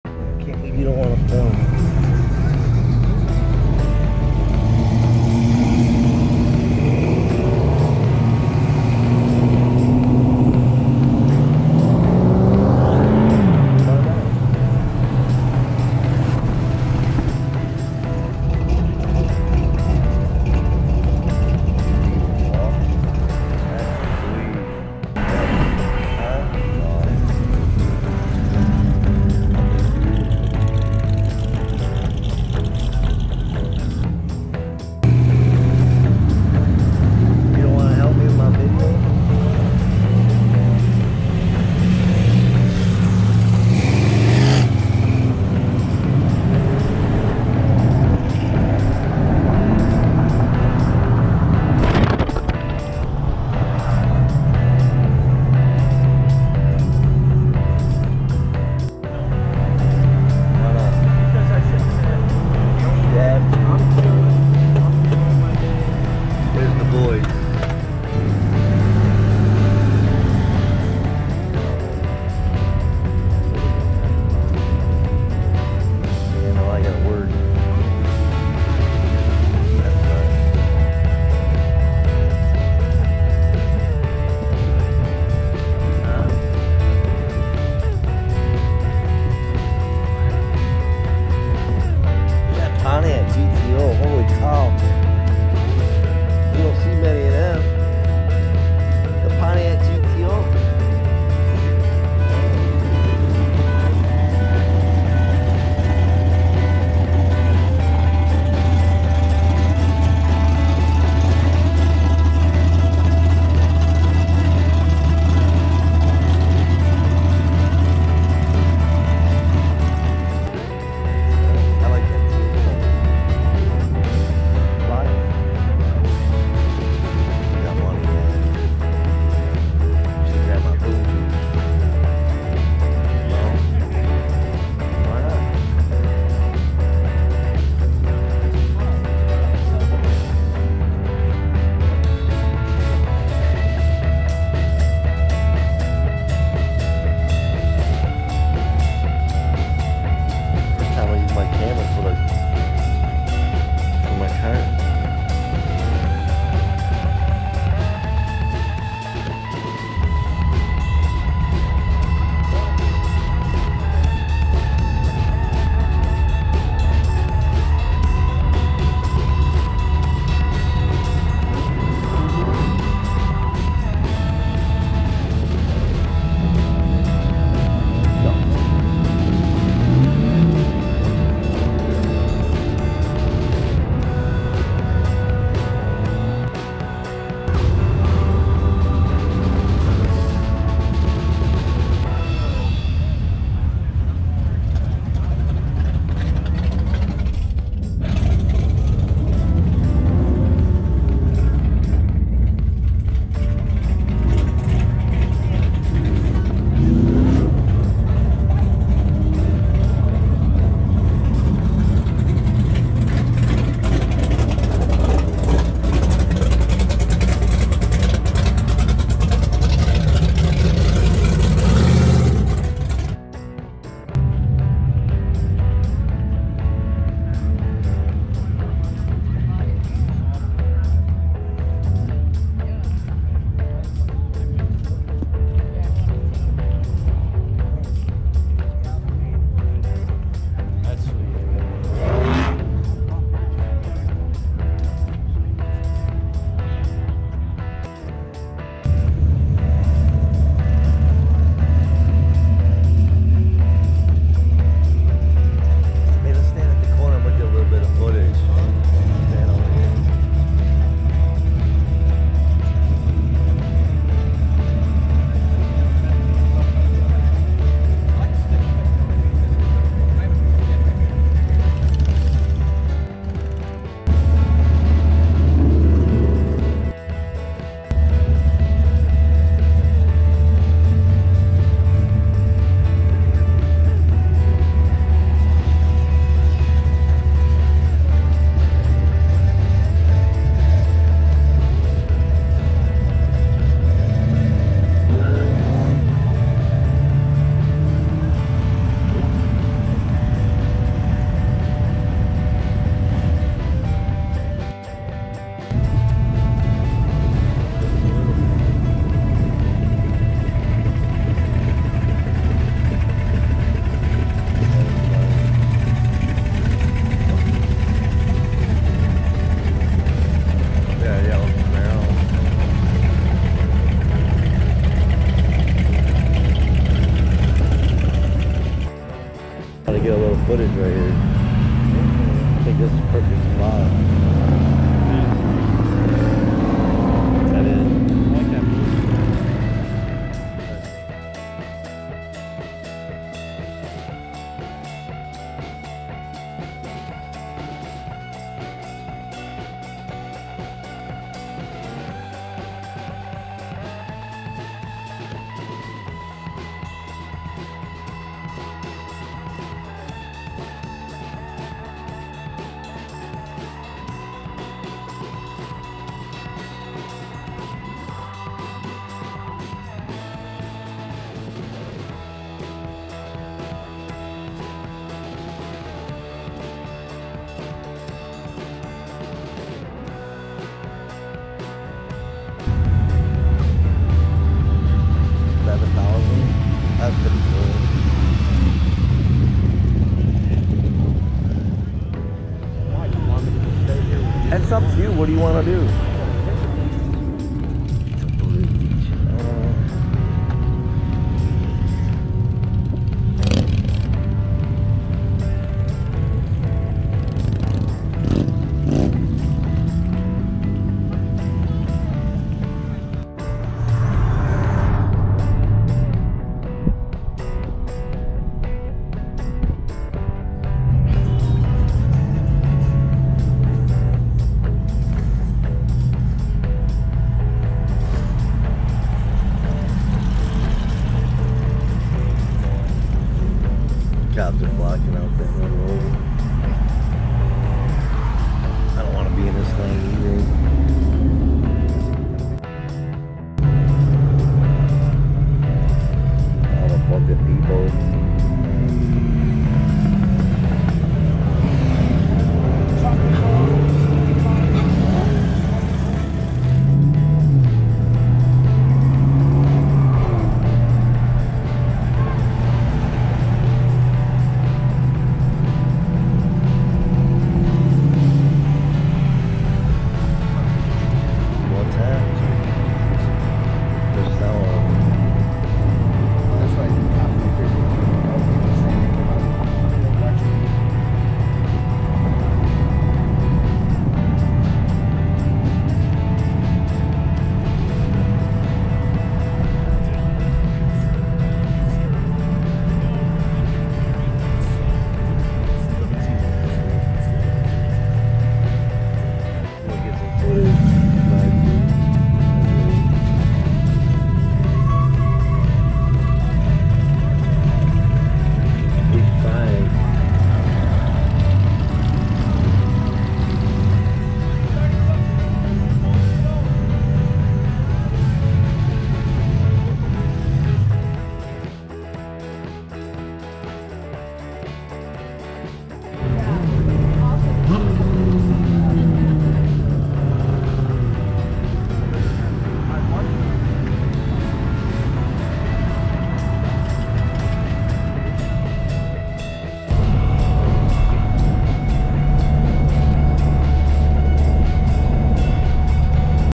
"Downriver Cruise" Friday 2019
Downriver Cruise 2019 - Cruise Fort Street to check out classic and muscle cars that are cruising and parked along Fort Street .

*Adam's Interior Detailer

Jax Wax Total Interior Cleaner

Natural Boar Hair Detail Brush (Set of 6)

*P&S Detailing Products - Bead Maker
*WOLFGANG Uber Rinse Less Wash

This hobby is supposed to be your therapy , not the reason to need therapy . Reflect your passion